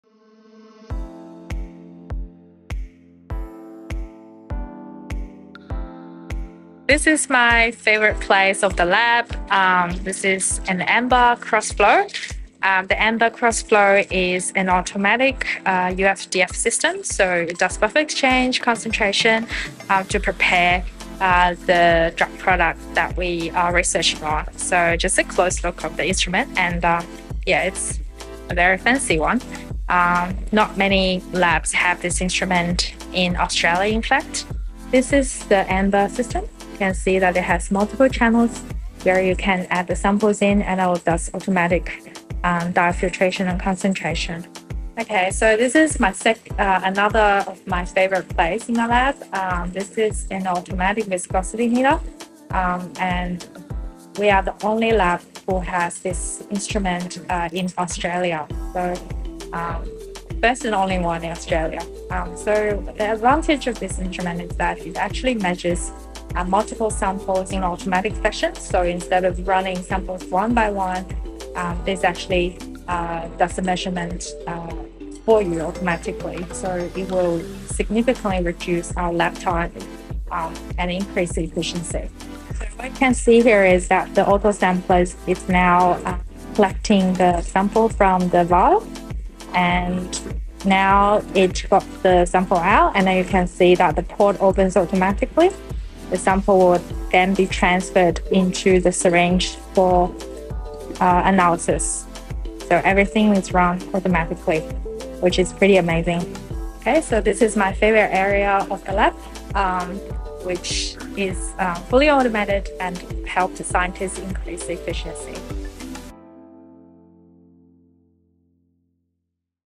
A Day in the Life of a Scientist at CSL - Danzi Song
Take a behind the scenes look at Danzi Song,  R&D Scientist at CSL, putting together life-saving solutions and revolutionising the way R&D works.